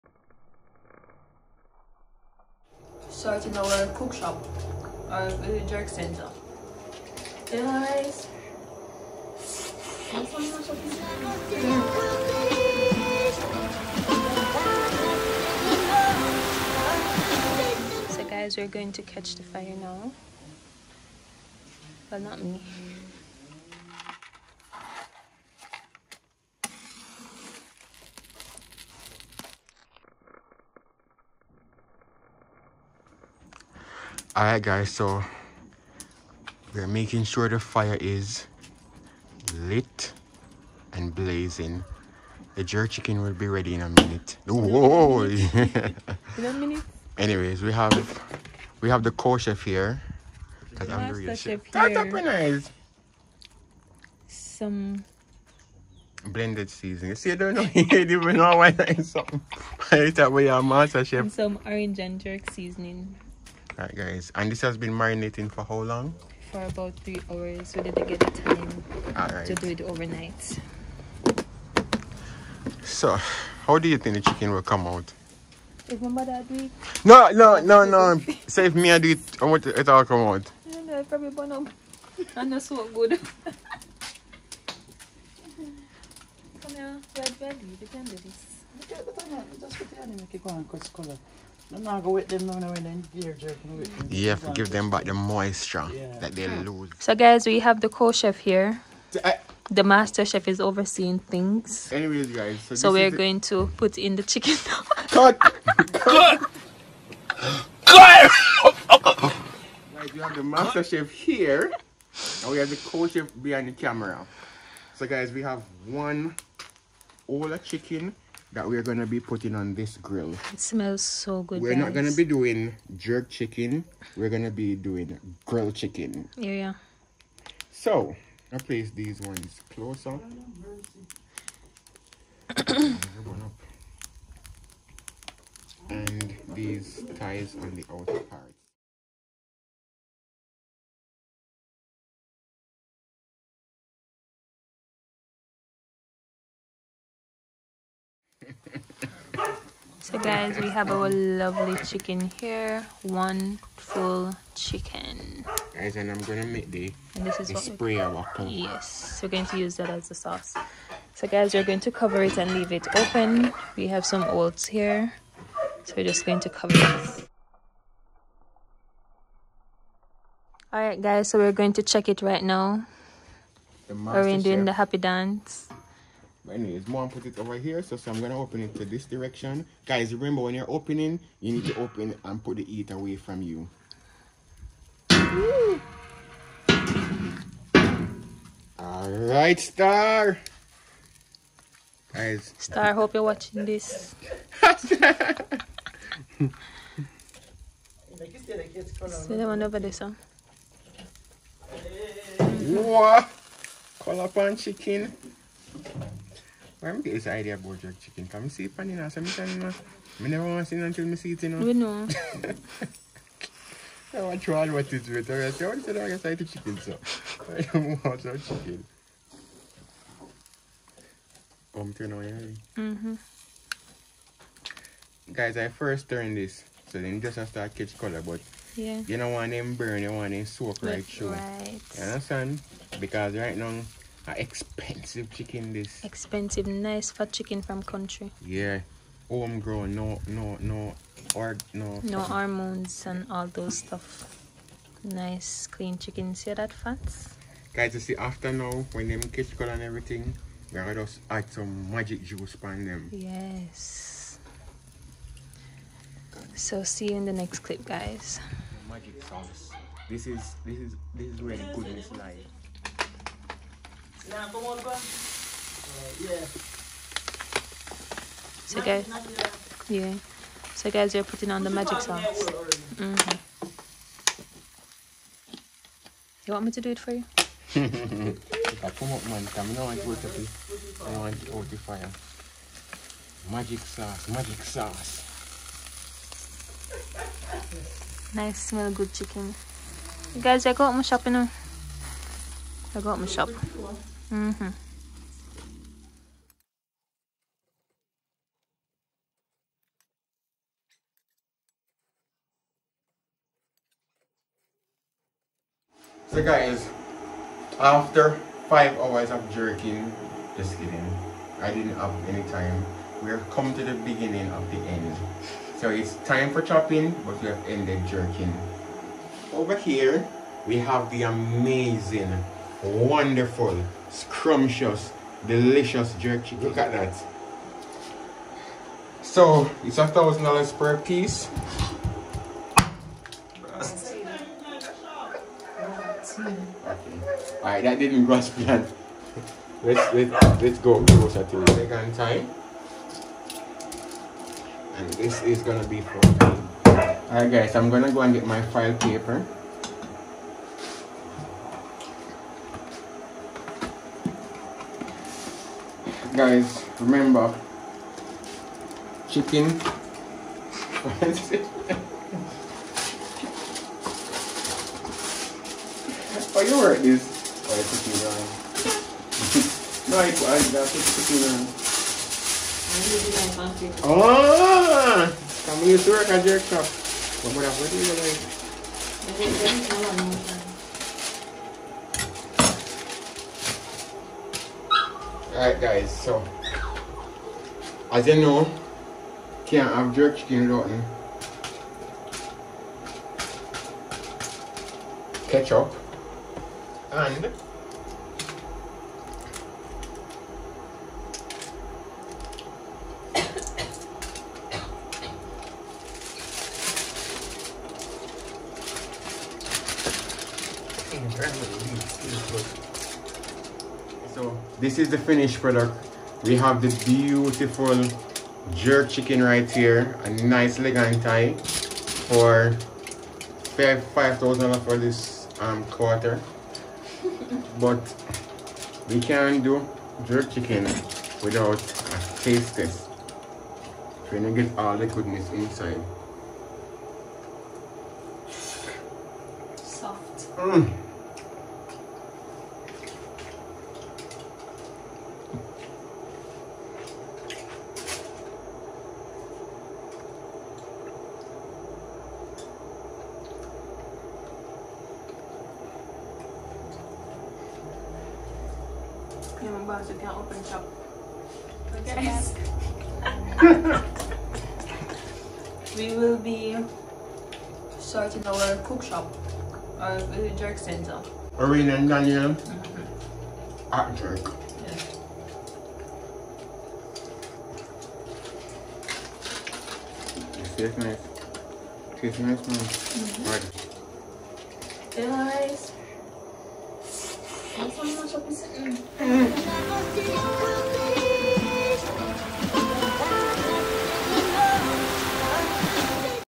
Jamaican Couple Cooking Authentic Jamaican Jerk Chicken
Join us as we jerk some nice succulent chicken.
Watch the Ads!!